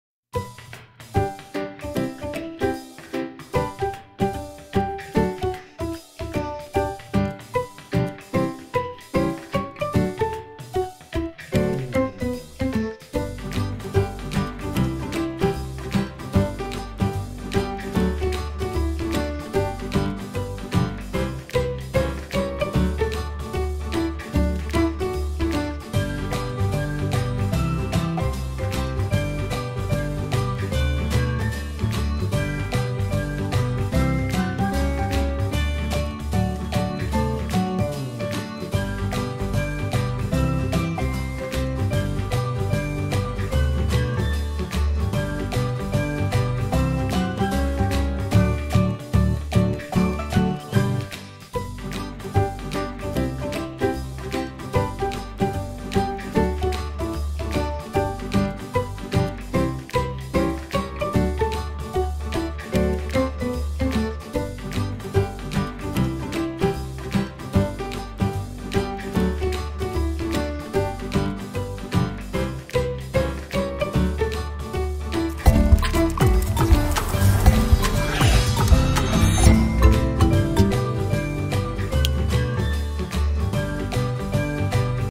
The Lifecycle of a Computer || Computer Aid International
Find out how you can make a difference to the world by using our secure IT disposal service to safely donate your organisation's computers!